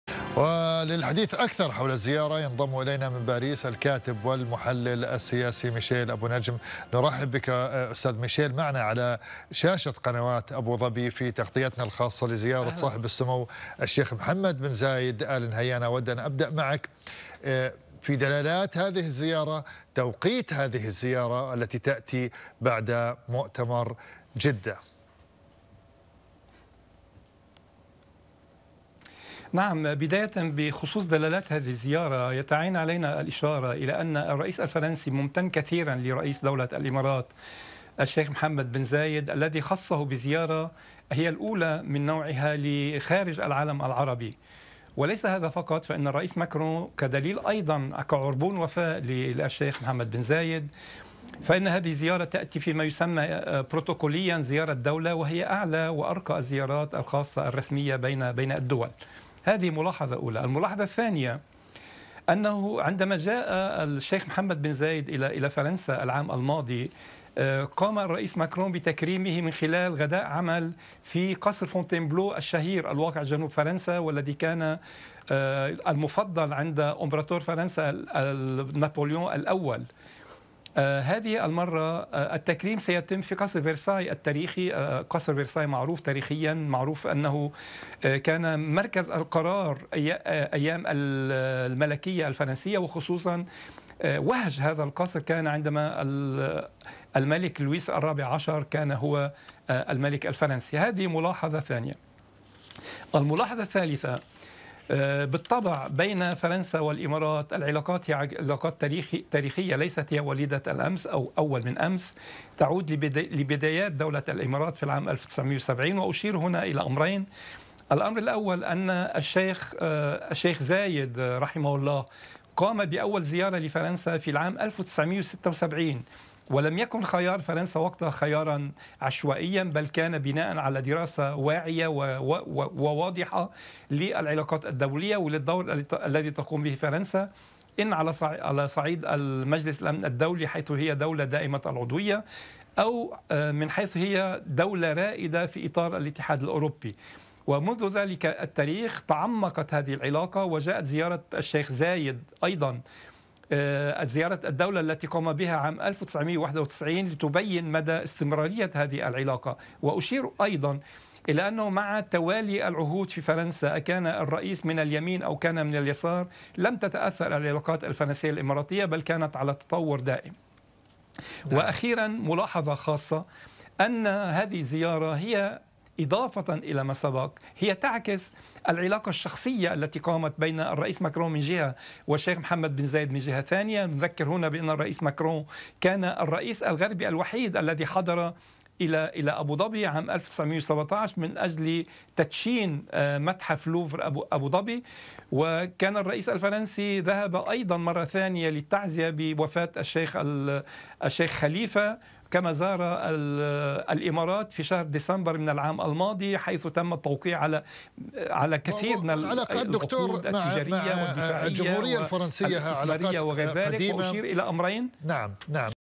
هذه الزيارة هي الأولى من نوعها خارج العالم العربي وتعتبر من أرقى الزيارات الرسمية بين الدول
ميشال أبو النجم: هذه الزيارة هي الأولى من نوعها خارج العالم العربي وتعتبر من أرقى الزيارات الرسمية بين الدول